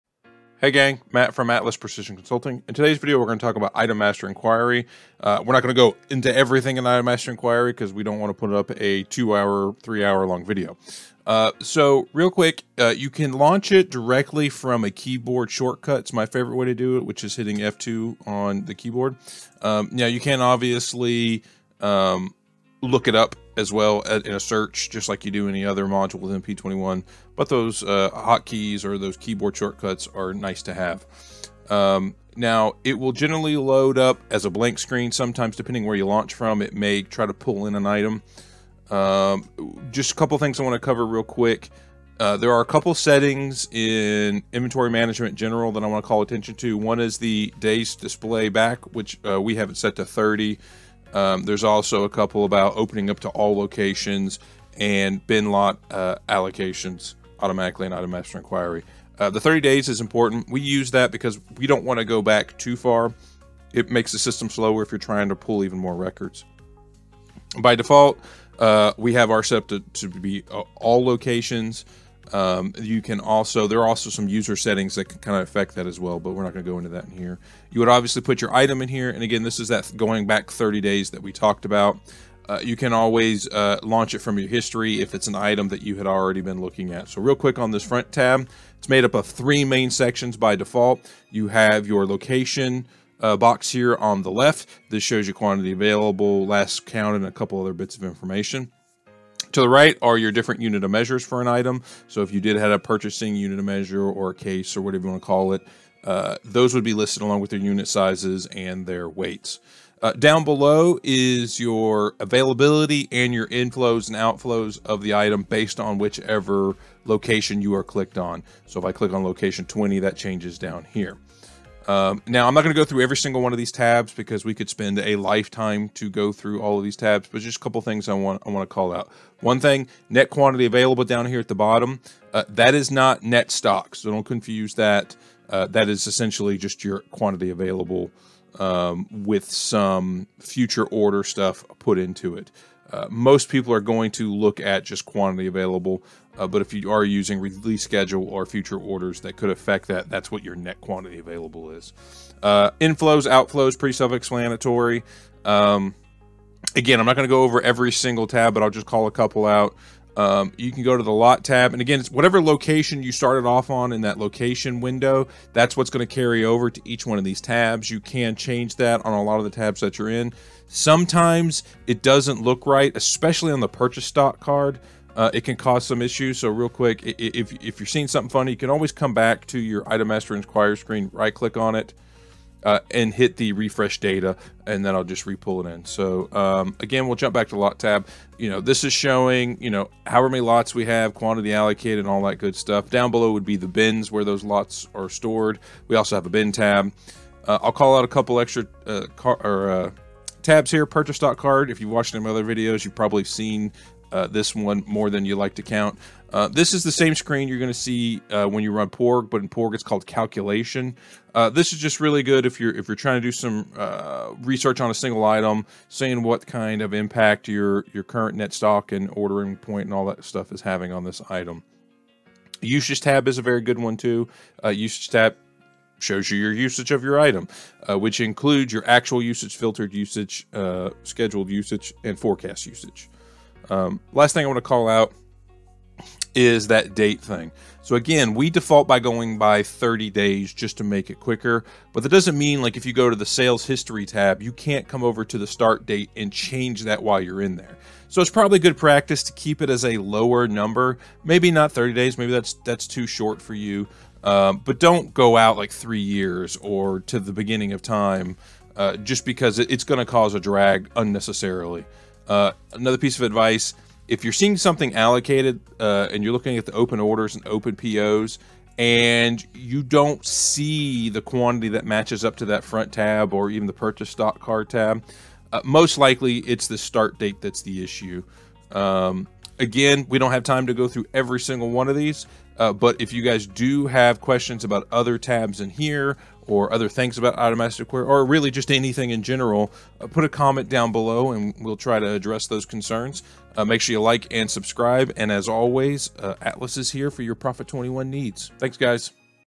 Prophet 21 Training and How To - Inventory: Item Master Inquiry
Inventory: Item Master Inquiry

⏱️ Timestamps:
0:00 Introduction
0:12 Launching IMI using F2
0:42 System Settings for IMI
1:22 Inventory in IMI
2:09 Net Quantity Available
2:40 Lot Tab
3:21 Purchase Stock Card
3:42 Usage Tab
3:54 Sales History
4:39 Conclusion